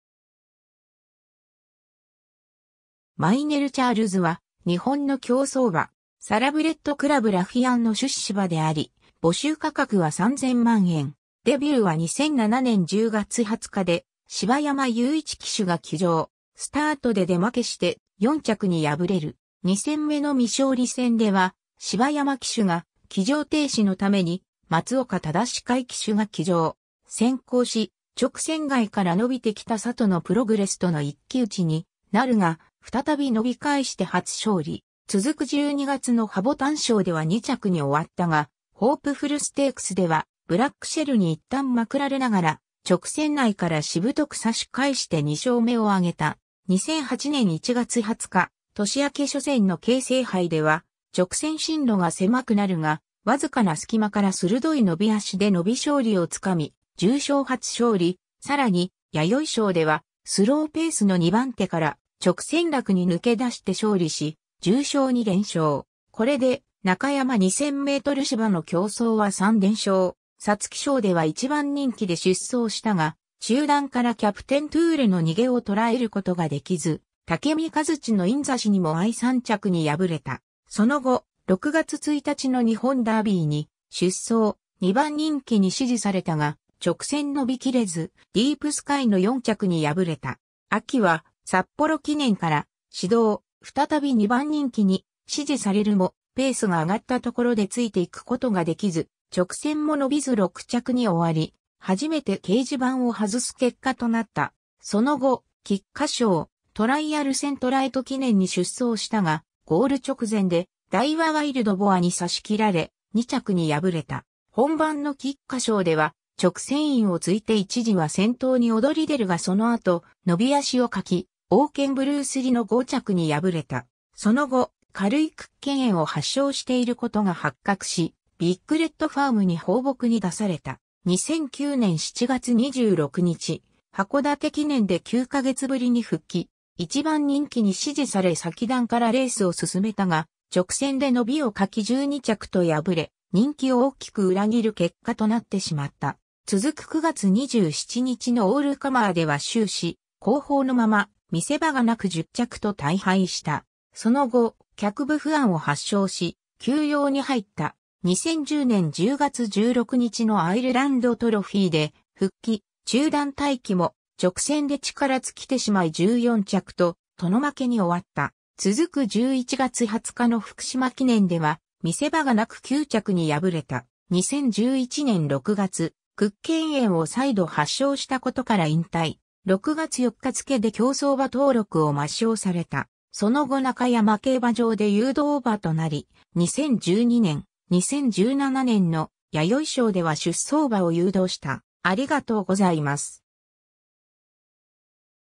マイネルチャールズ
マイネルチャールズは日本の競走馬。
サラブレッドクラブ・ラフィアンの出資馬であり、募集価格は3000万円（15万円×200口）。
 デビューは2007年10月20日で柴山雄一騎手が騎乗。
スタートで出負けして、4着に敗れる。
2戦目の未勝利戦では、柴山騎手が騎乗停止のために松岡正海騎手が騎乗。
先行し、直線外から伸びてきたサトノプログレスとの一騎討ちになるが、再び伸び返して初勝利。
続く12月の葉牡丹賞では2着に終わったが、ホープフルステークスではブラックシェルに一旦まくられながら直線内からしぶとく差し返して2勝目を挙げた。
 2008年1月20日、年明け初戦の京成杯では直線進路が狭くなるが、わずかな隙間から鋭い伸び脚で伸び勝利を掴み重賞初勝利、さらに弥生賞では、スローペースの2番手から直線楽に抜け出して勝利し重賞2連勝。
これで中山2000メートル芝の競走は3連勝。
皐月賞では1番人気で出走したが、中団からキャプテントゥーレの逃げを捉えることができず、タケミカヅチのイン差しにも遭い3着に敗れた。
その後、6月1日の日本ダービーに出走、2番人気に支持されたが、直線伸びきれず、ディープスカイの4着に敗れた。
秋は札幌記念から始動、再び2番人気に支持されるも、ペースが上がったところでついていくことができず、直線も伸びず6着に終わり、初めて掲示板を外す結果となった。
その後、菊花賞トライアルセントライト記念に出走したが、ゴール直前でダイワワイルドボアに差し切られ、2着に敗れた。
本番の菊花賞では直線インを突いて一時は先頭に躍り出るがそのあと伸び脚を欠き、オウケンブルースリの5着に敗れた。
その後、軽い屈腱炎を発症していることが発覚し、ビッグレッドファームに放牧に出された。
 2009年7月26日、函館記念で9ヶ月ぶりに復帰。
1番人気に支持され先団からレースを進めたが、直線で伸びを欠き12着と敗れ、人気を大きく裏切る結果となってしまった。
続く9月27日のオールカマーでは終始後方のまま見せ場がなく10着と大敗した。
その後、脚部不安を発症し休養に入った。
 2010年10月16日のアイルランドトロフィーで復帰、中団待機も直線で力尽きてしまい14着と殿負けに終わった。
続く11月20日の福島記念では見せ場がなく9着に敗れた。
 2011年6月、屈腱炎を再度発症したことから引退。
6月4日付けで競走馬登録を抹消された。
その後中山競馬場で誘導馬となり、2012年,2017年の弥生賞では出走馬を誘導した。